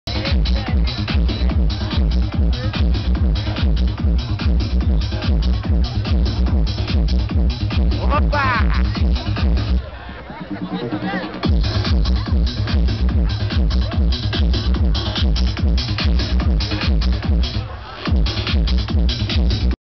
Dj Feio @ XXXperience, Curitiba 2006
Amazing Rave with all my buddies from Australia, USA, Mexico and of course, Brasiiiilllll !!!